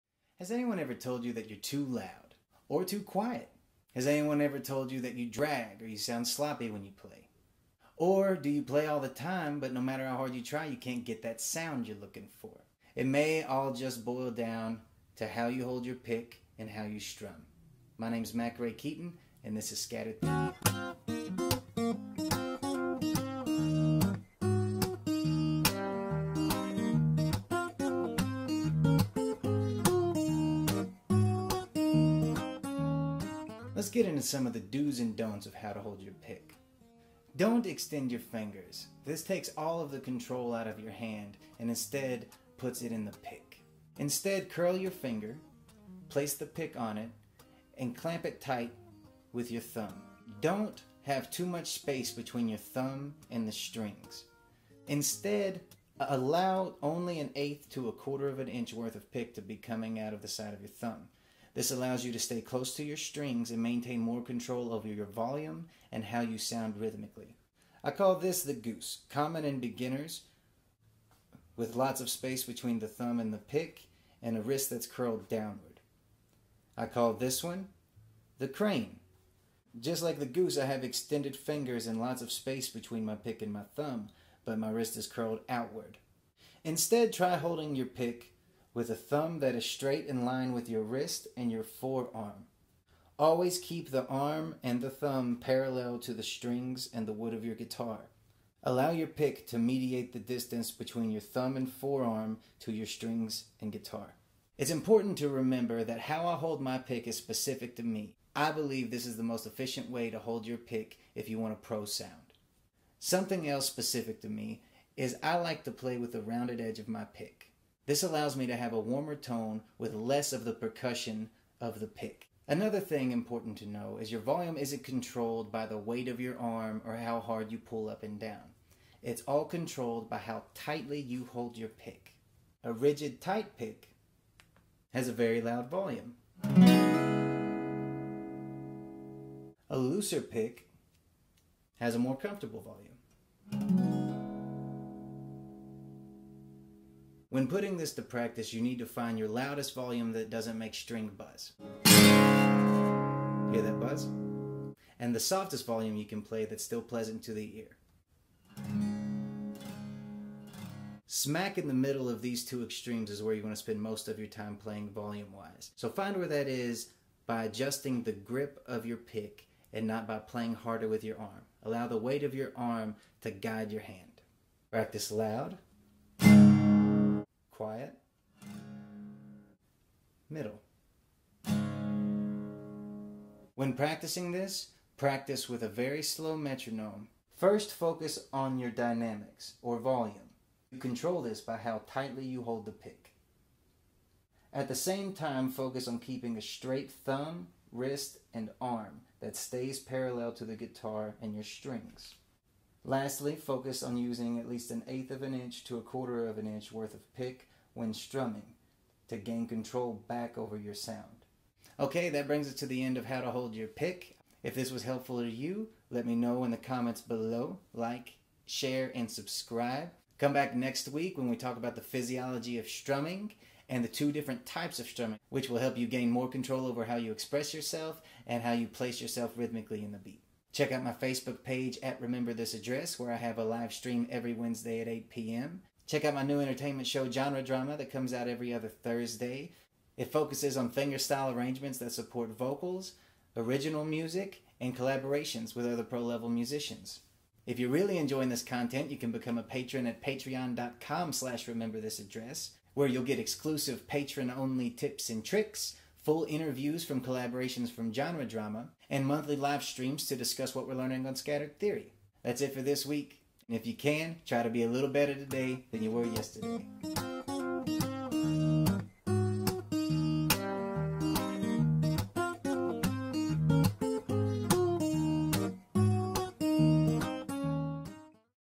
Something I wish every guitar player knew - How To Hold Your Guitar Pick - BEGINNER GUITAR LESSON
Scattered Theory Ep. 1 - How to Hold Your Pick
Month 1
Right Hand

As simple as it is, how to hold your guitar pick is a fundamental foundational block of pro technique. Learn to have real control over your volume and rhythm by making these adjustments!

Like me on the other socials!
Insta, Snap, TikTok
Mack Ray Keaton